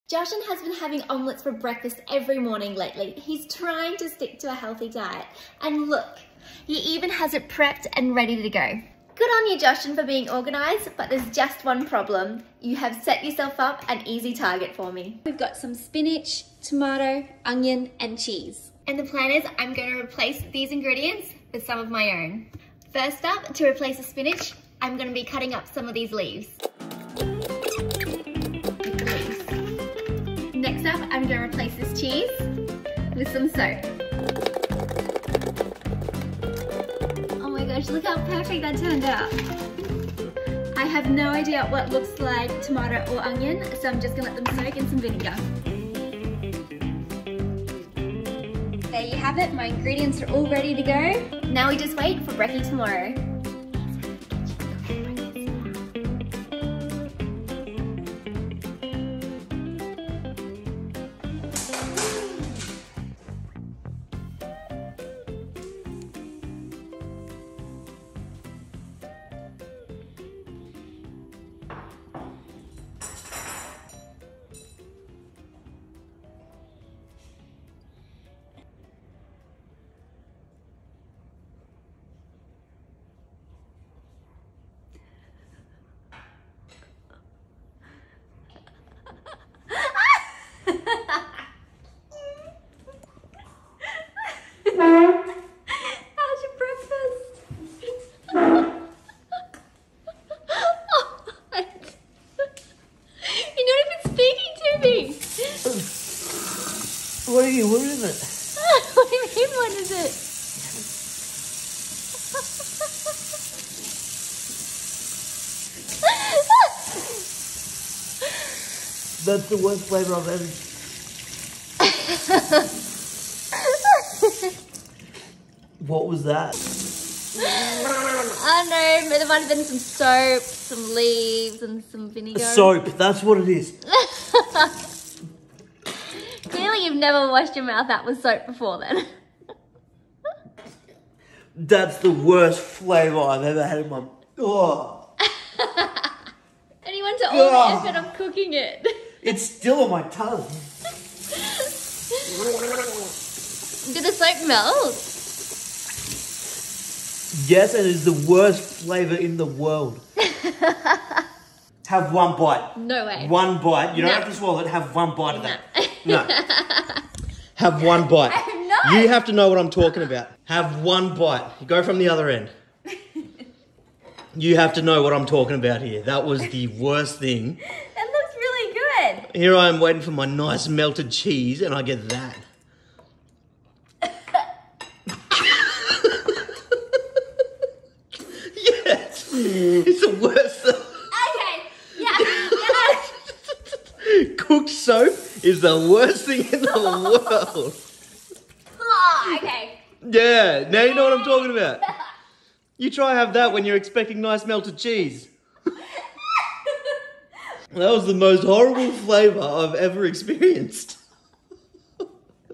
I Can't Believe How Bad this Was!
Jashan & Cassandra's Husband vs wife prank war.
Cassandra gets into Jashan's meal prep with a little surprise.